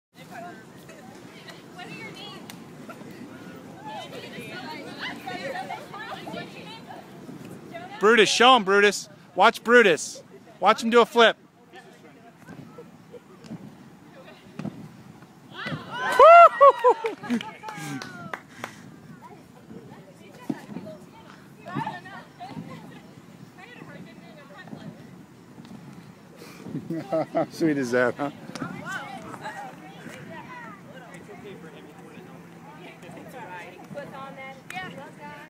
Brutus jumps on the Jumping Pillow
Brutus the Buckeye jumps on the Jumping Pillow at Mapleside Farms. The Jump Park was voted by Scene Magazine in 2012 as Clevelands best playground for kids!